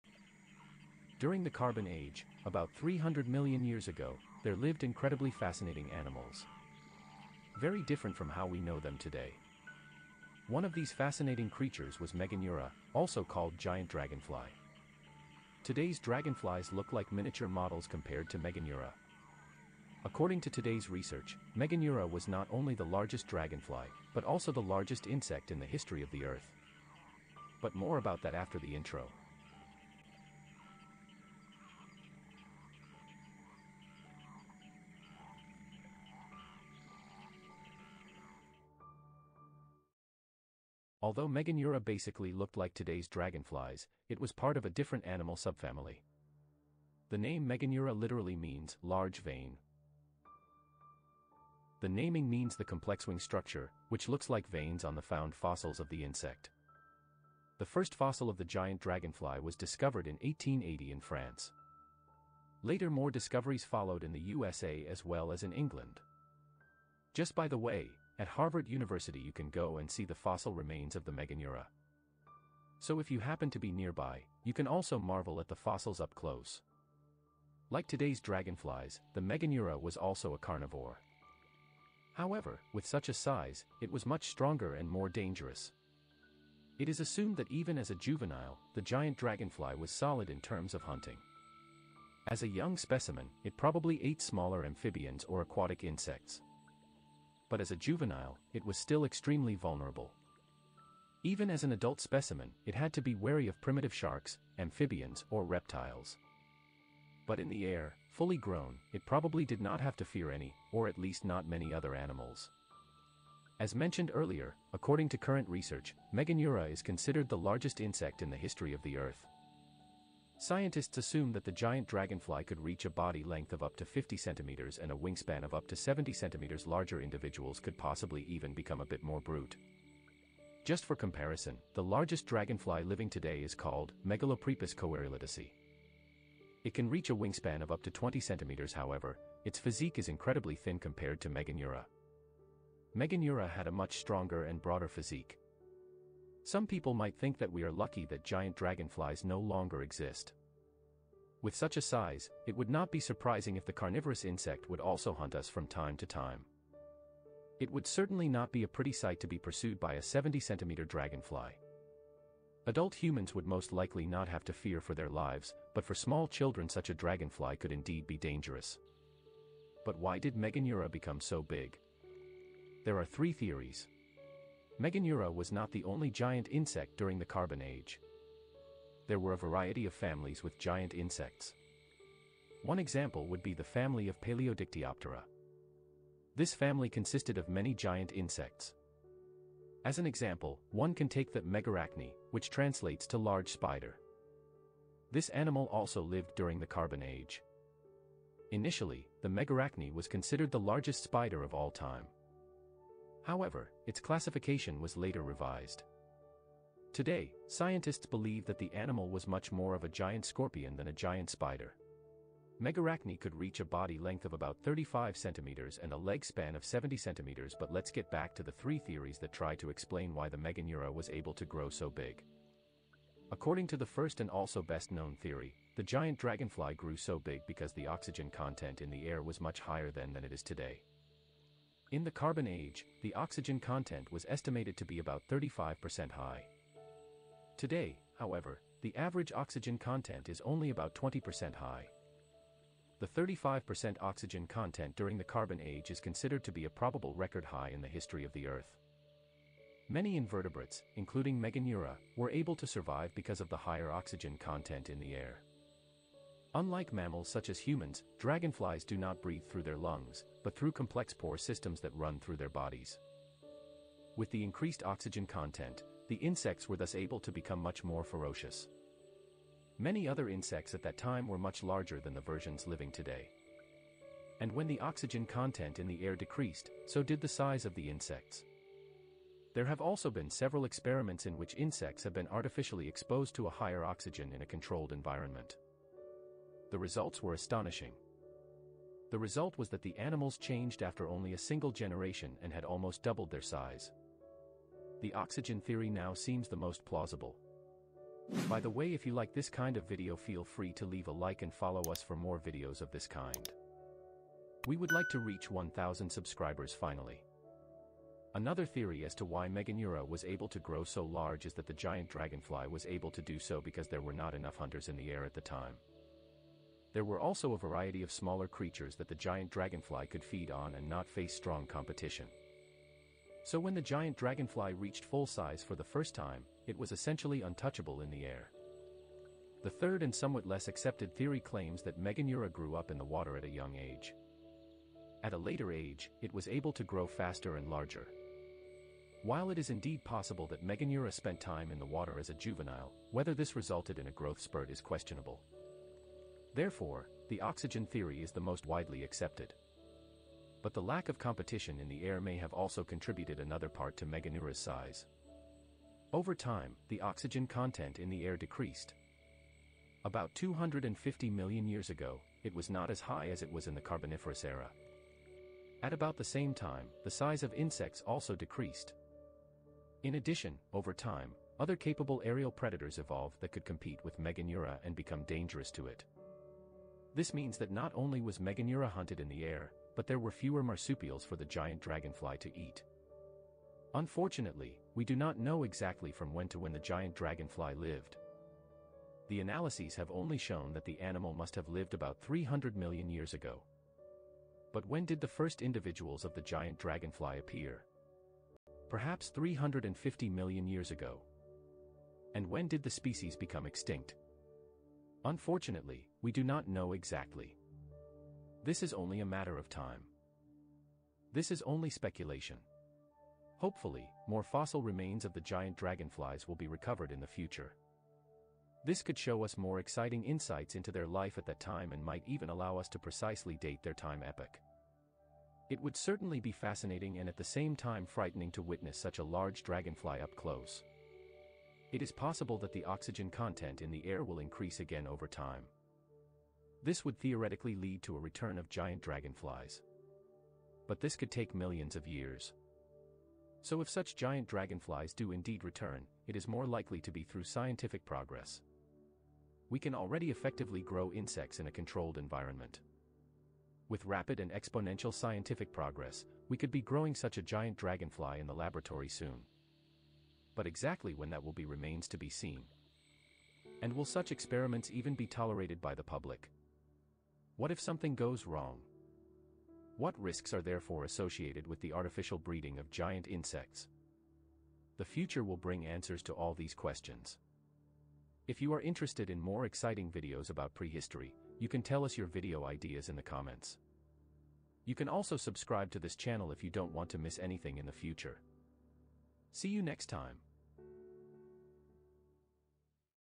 Meganeura - The Largest Insect In The History Of The World
In the history of our earth there have been many interesting creatures, especially before our lifetime. Meganeura is a giant dragonfly that has been in the air in the Carbon Age. They resembled and are related to the present-day dragonflies and damselflies, and were predatory, with their diet mainly consisting of other insects.